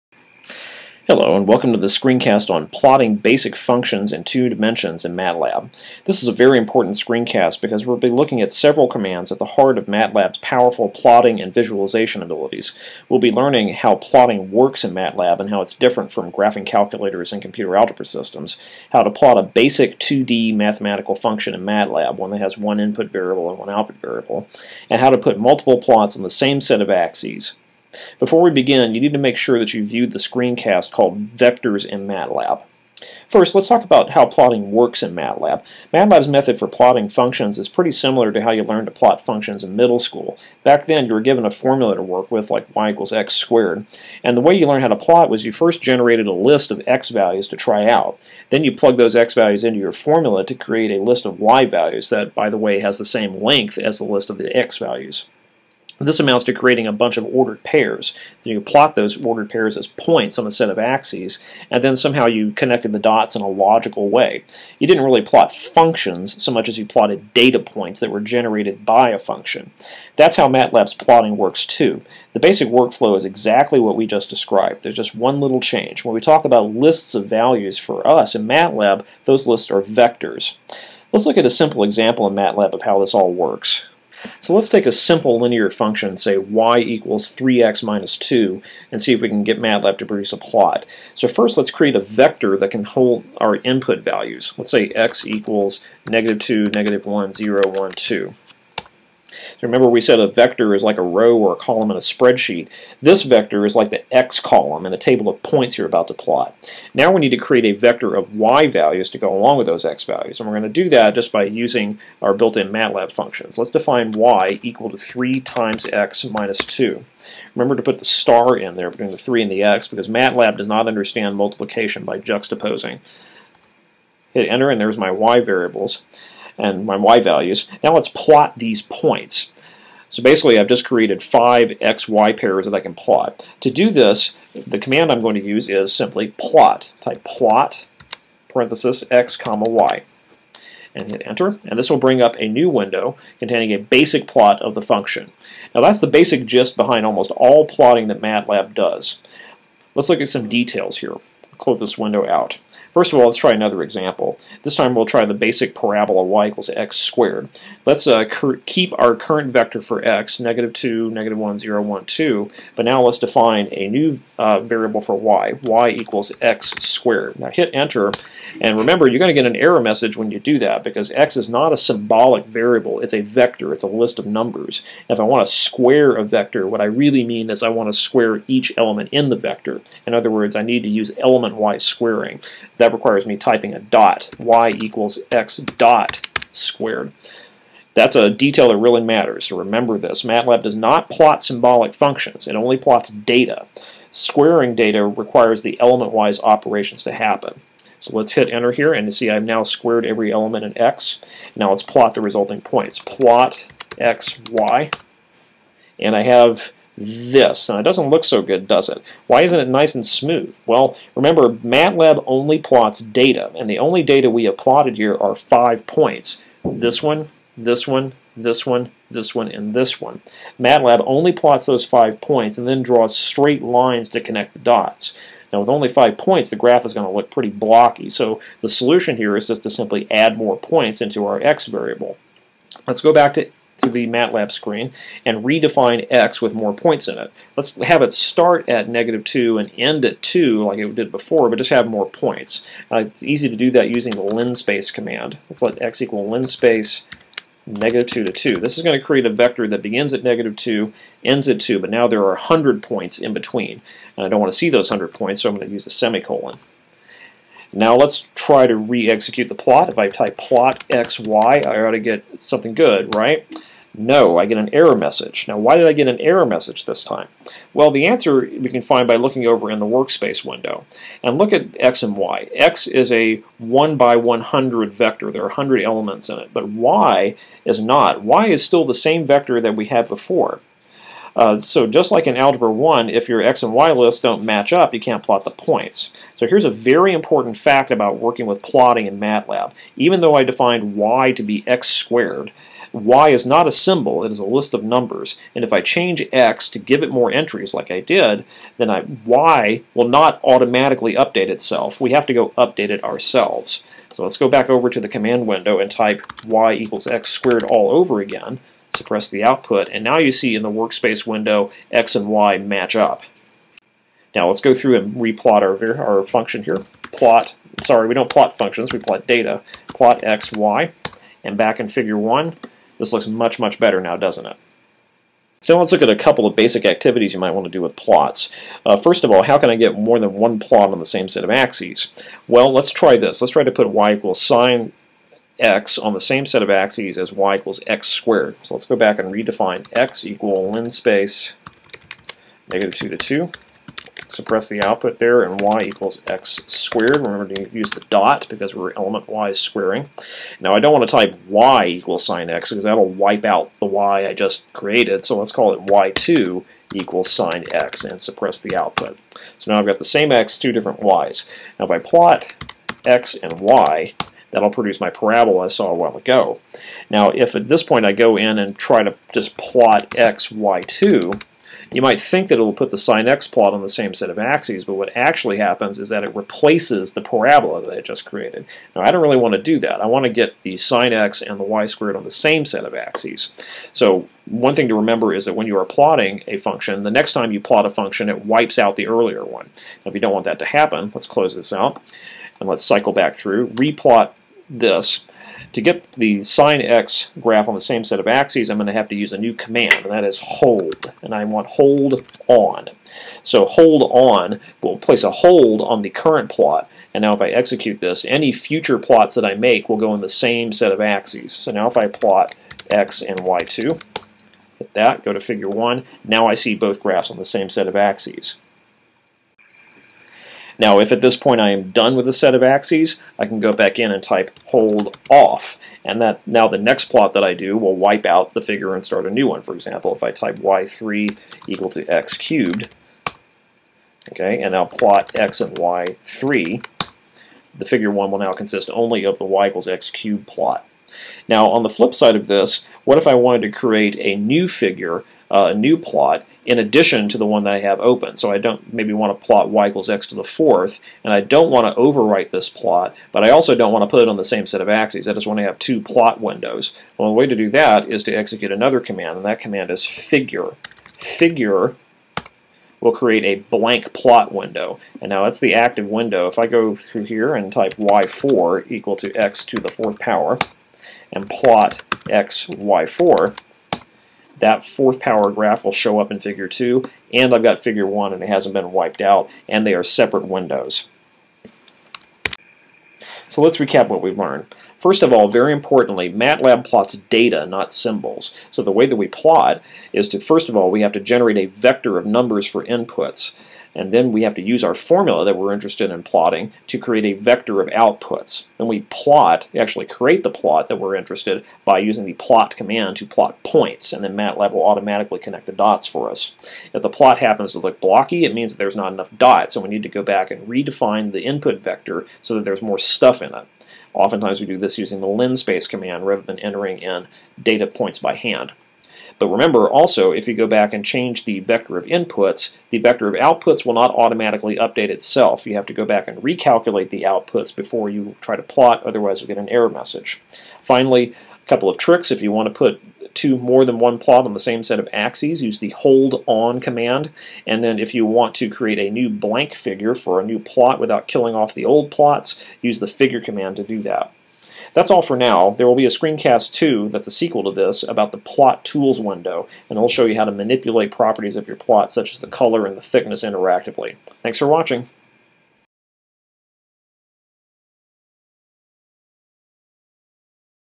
MATLAB Function Plotting Basics
Screencast on the basics of plotting one-variable (two-dimensional) functions in MATLAB. Discusses the philosophy of how MATLAB makes plots, the PLOT function, potential pitfalls when making plots, and the HOLD ON/HOLD OFF and FIGURE commands.